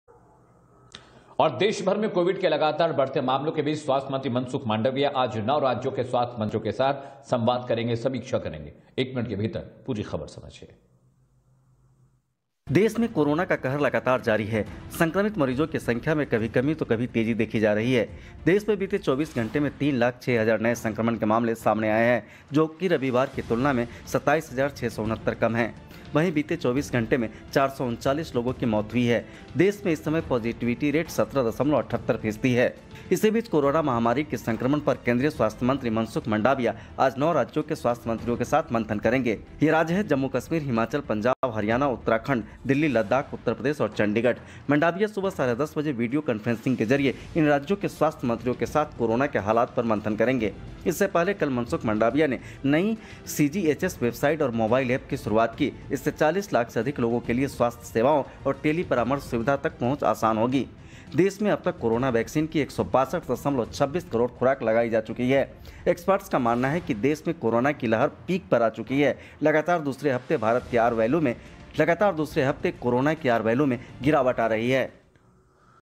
Corona के मद्देनजर स्वास्थ्य मंत्री Mansukh Mandaviya आज Delhi समेत 9 राज्यों के साथ करेंगे मंथन
देशभर में Corona कहर लगातार जारी है। संक्रमितों की संख्या कभी कम तो कभी तेजी देखी जा रही है। इसी के मद्देनजर स्वास्थ्य मंत्री Mansukh Mandaviya Delhi, Punjab समेत 9 राज्यों कं साथ कोरोना की स्थिति की समीक्षा करेंगे।